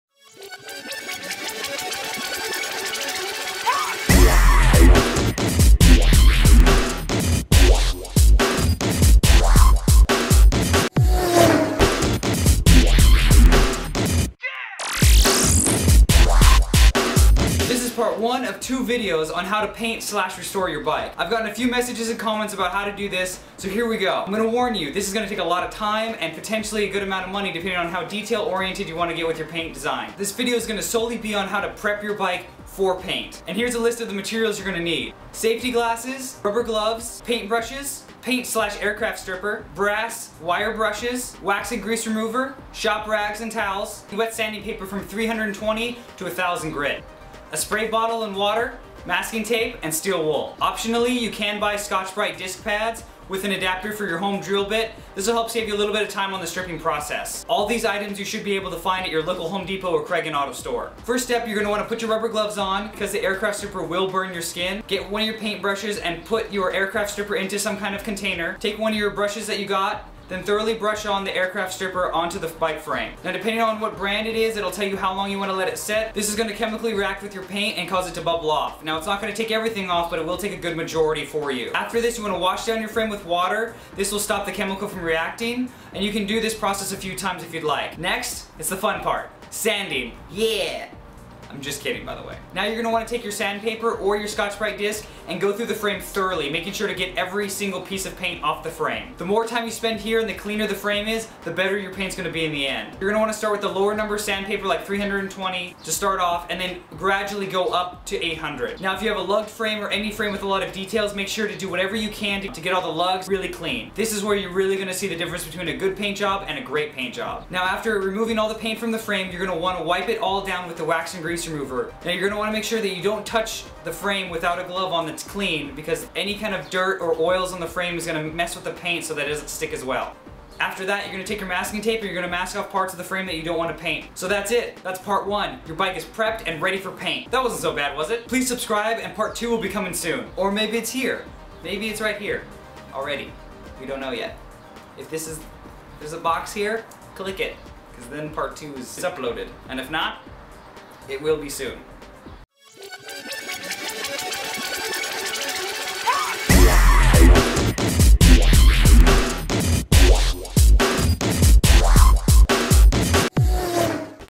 How to Paint / Restore Your Bike Frame Part 1
This videos is Part 1 on how to prep your bike for paint

Tools you need:
Saftey glasses
paint brush
Rubber gloves
Paint / aircraft Stripper
Wire Brushes
Wax and grease remover
shop rags
wet sanding Sandpaper from 320 grit to 1000
spray bottle with water
Masking tape
steel wool

Optional
Getting scotch bright sanding disks with adapter for your drill at home